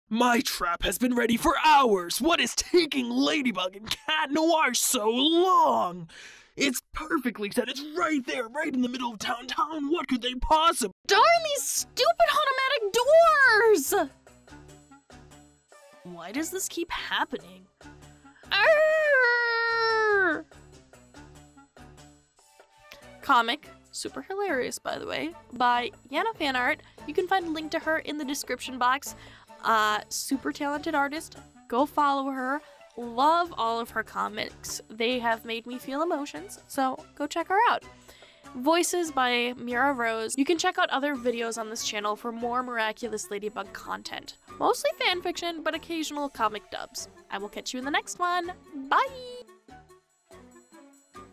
Heroes vs Automatic Doors (LadyNoir, Comic, Hawkmoth Frustrations) A Miraculous Ladybug Comic Dub
Ladybug and Cat Noir are delayed by an old foe... That isn't Hawkmoth? - A Miraculous Ladybug Comic dubbed in English

Voiced by Mira Rose

Want to support me? You can do so by following these links!